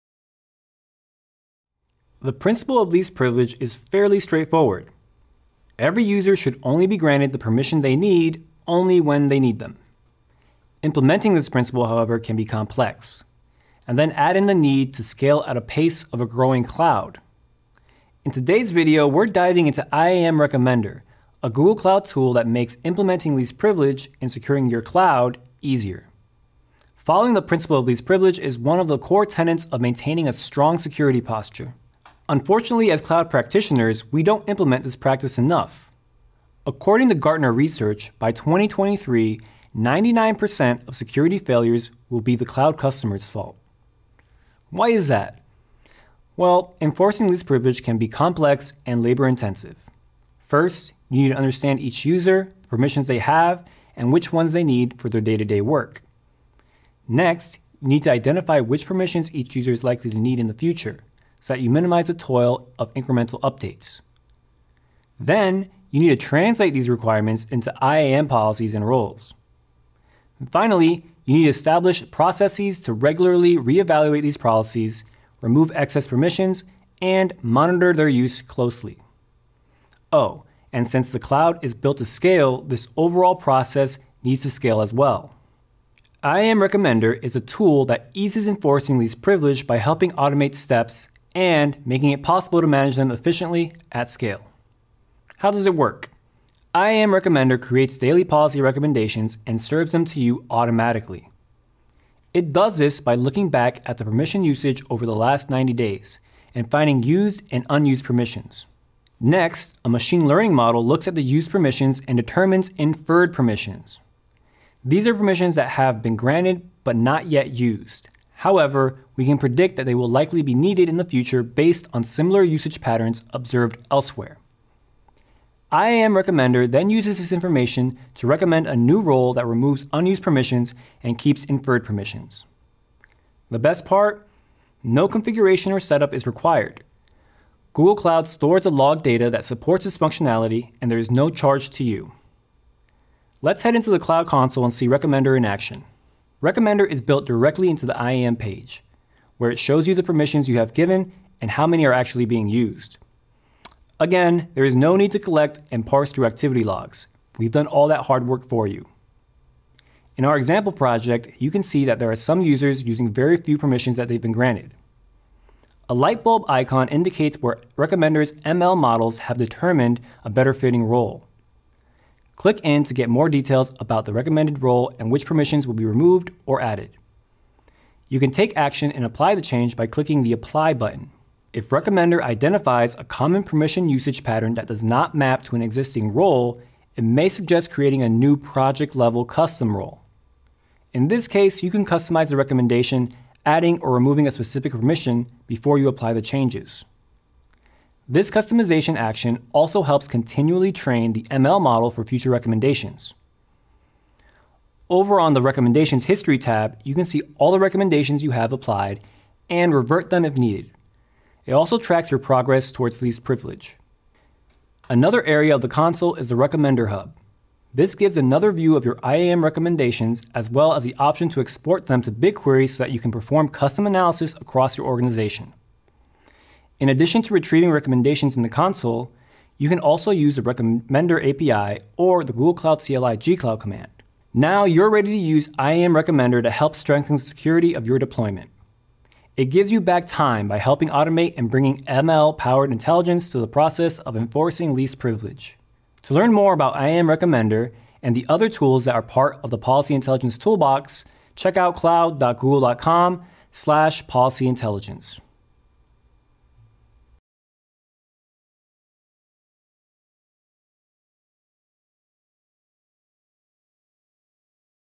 What is IAM Recommender?
Curious about the principle of least privilege? Want to learn how to implement this principle at scale? In this episode of Optimize with Active Assist, we cover IAM Recommender: a Google Cloud tool that makes it easier to secure your cloud. Watch to learn how you can use IAM Recommender!

Chapters:
0:00 - Intro
0:26 - The principle of least privilege
0:50 - Implementing least privilege
1:26 - What is IAM Recommender?
1:37 - How does IAM Recommender work?
2:26 - Setting up IAM Recommender in the cloud console
3:53 - Wrap up